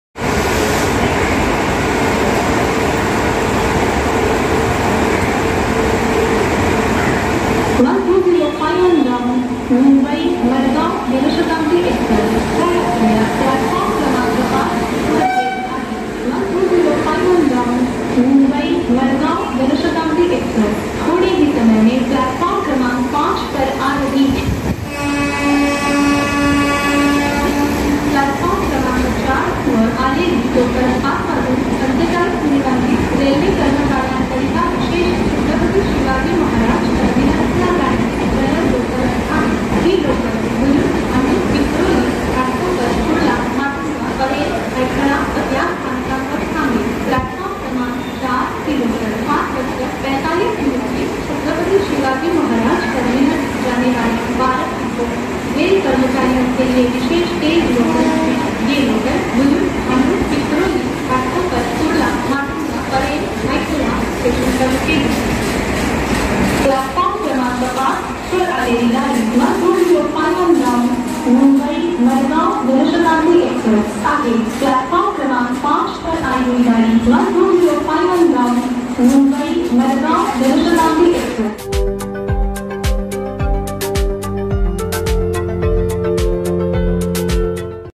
Mumbai Madgaon Jan Shatabdi Express Arriving At Thane | Konkan Railways
12051 DN Mumbai CSMT Madgaon Jan Shatabdi Express Arriving At Thane Railway Station Superbly...

Location : Thane, Maharashtra
Camera : Samsung Galaxy M31 S
Locomotive : Kalyan WAP-7